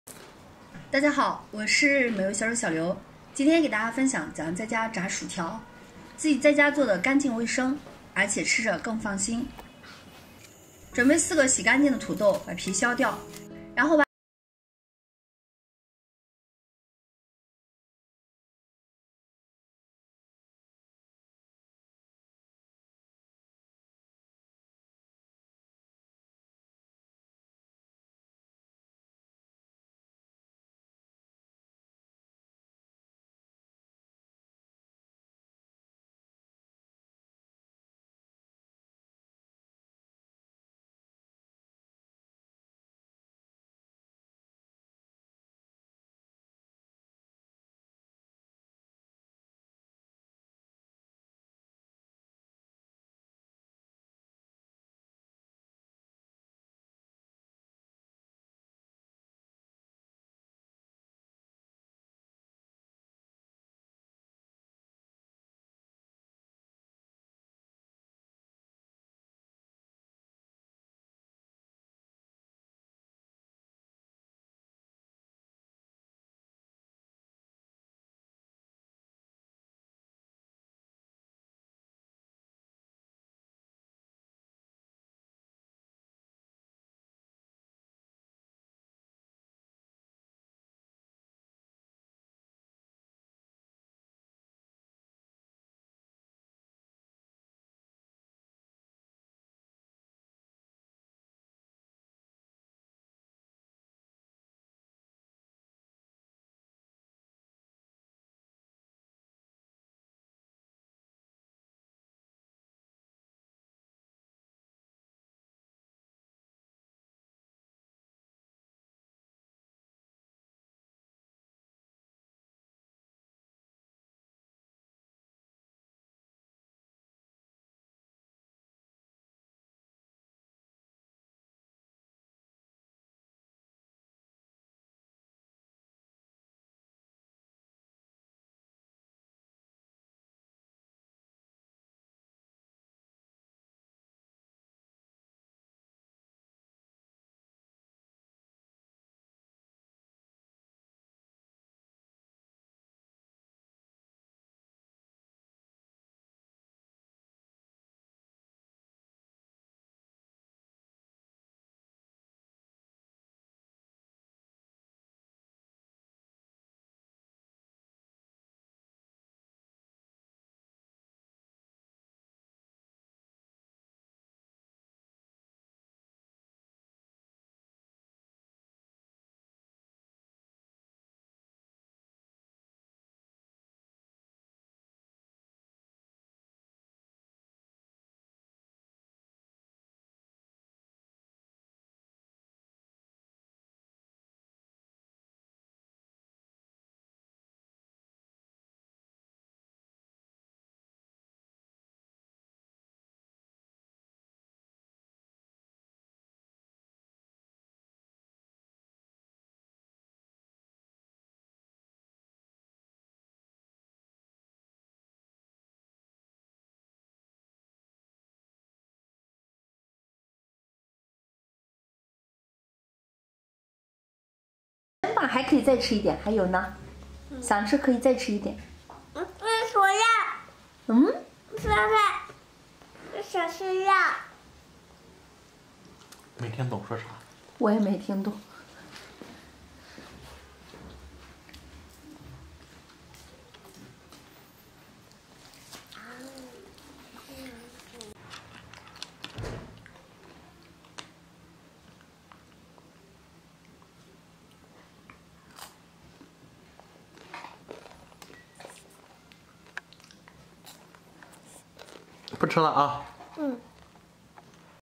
教你在家炸薯条，成本不到3块钱，一做一大盆，学会不用去外面吃
美味小舍 今天给大家分享怎样在家炸薯条，炸好的薯条色泽金黄酥脆好吃，就算凉了也不会变软。做法简单，掌握做法诀窍是关键，看一遍就能学会。